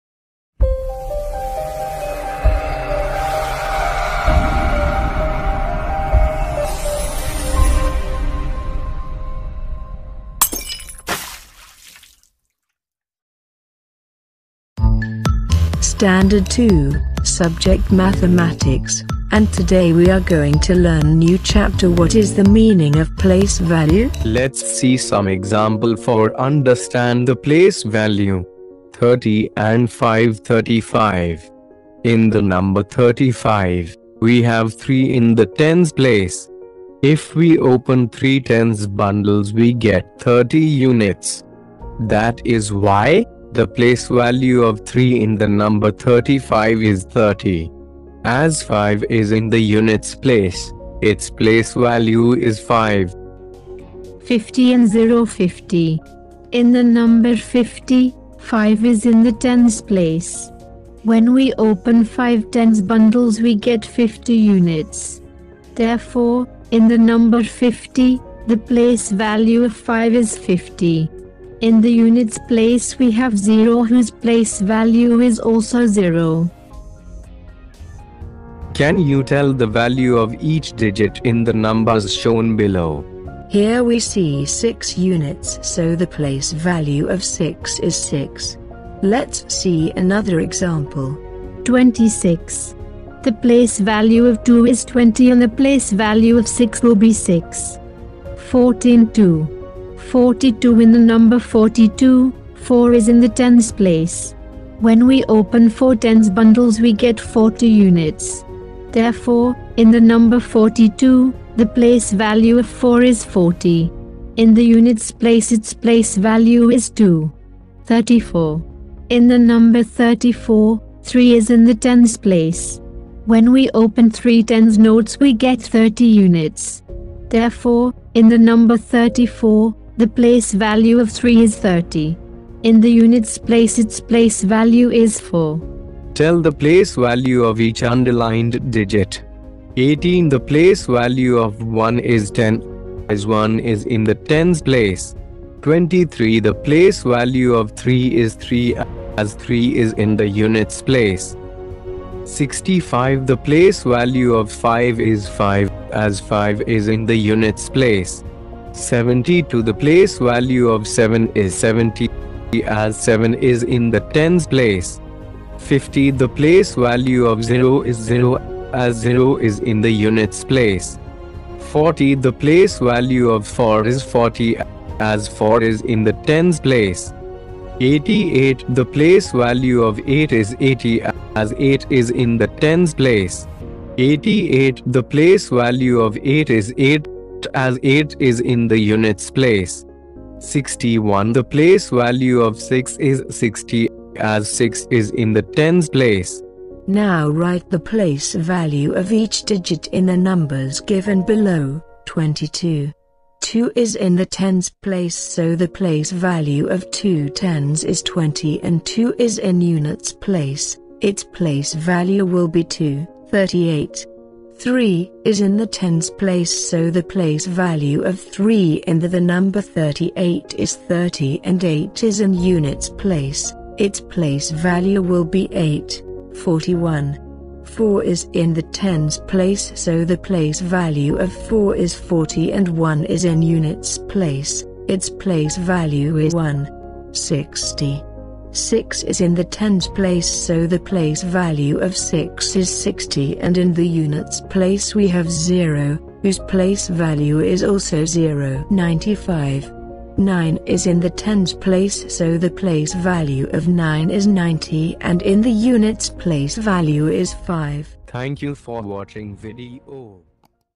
What is the meaning of 'place value'?|Std-2|Math
Place value-An explanation is given with examples.